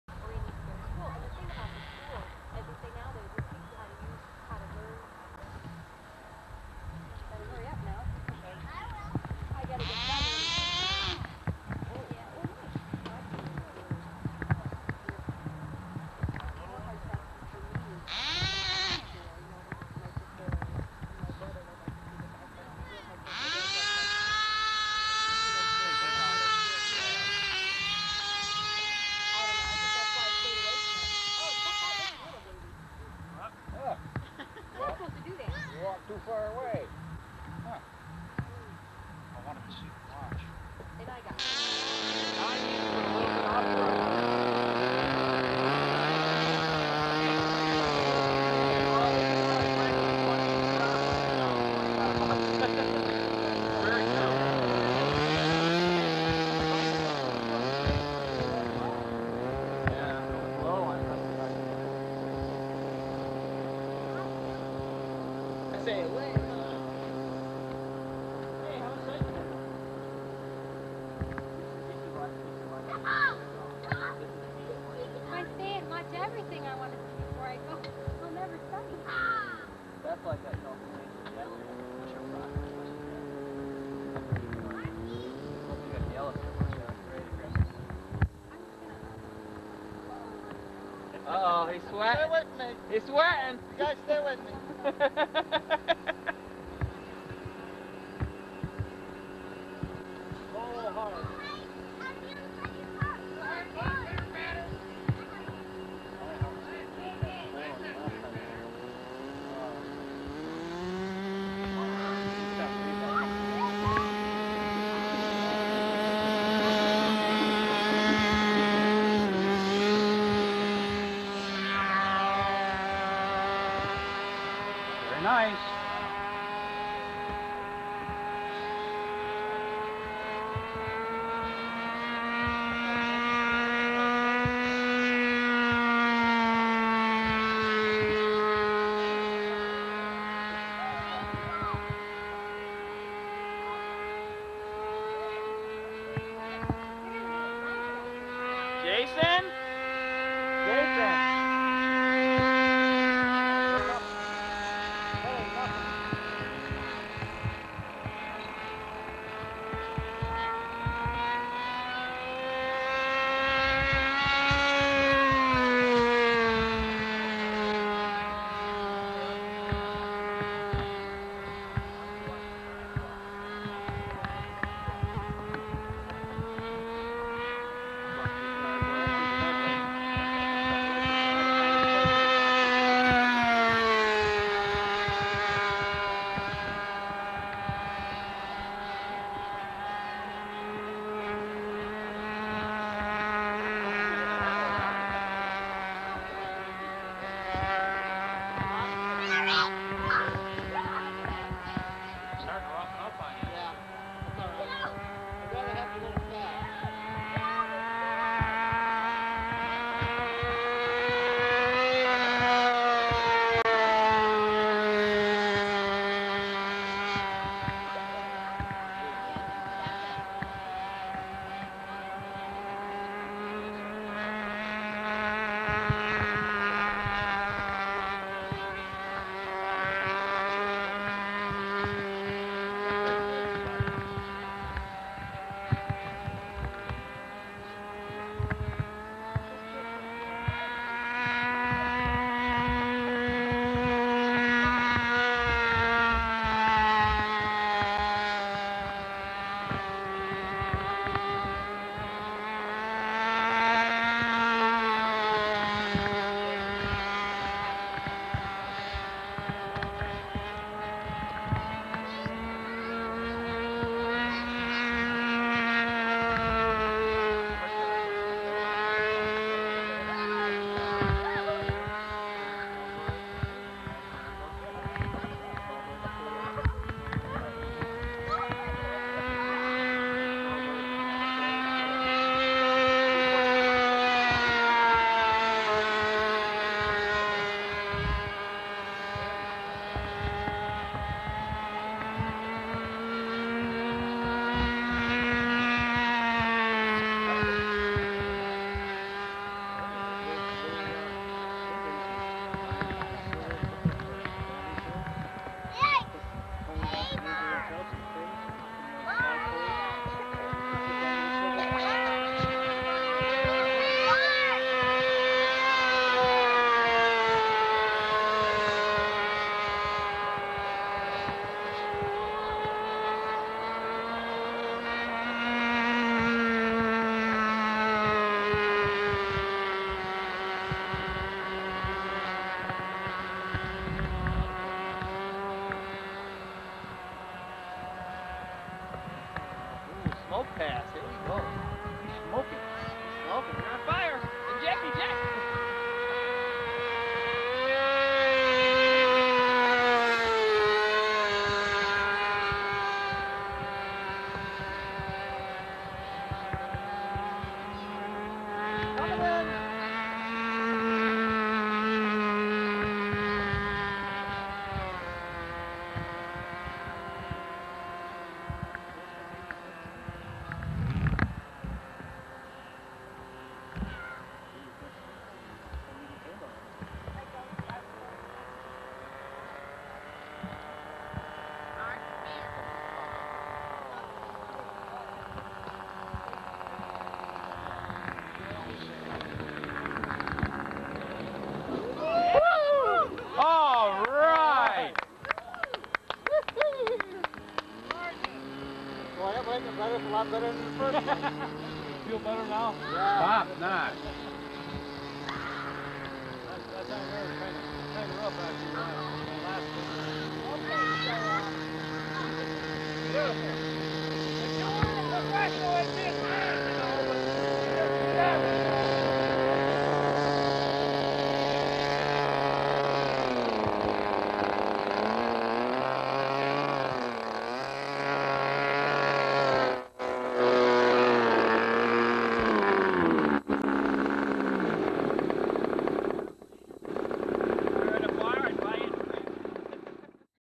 G&P Sales f 18 Pusher prop 1996
here's an OLD clip of my G&P sales f-18 pusher . using a rossi .60 blackhead for power. Shot in bath,ny at our fall picnic The B.A.R.K.S. club